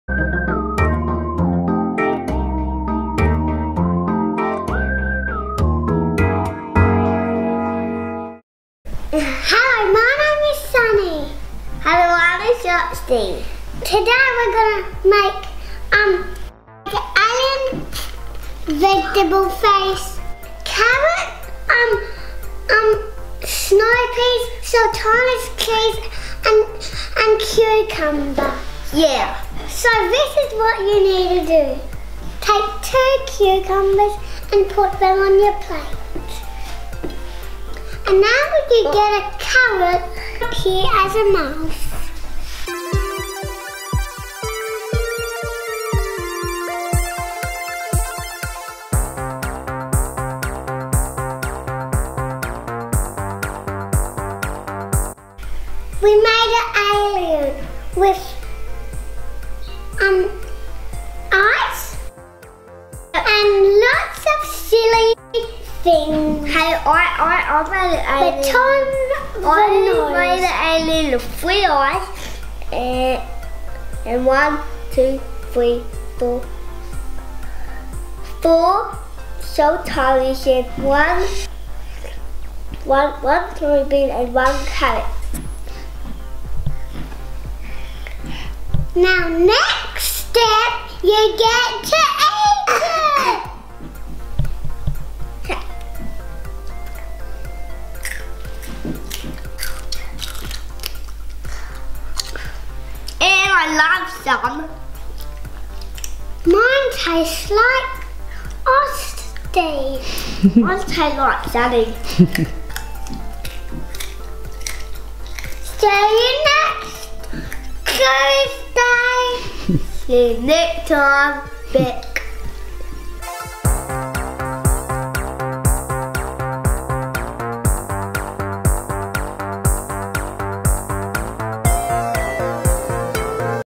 Kids Cooking - Austin and Sunny Make a Veggie Alien
Austin and Sunny show you how to make an alien face out of delicious fresh vegetables!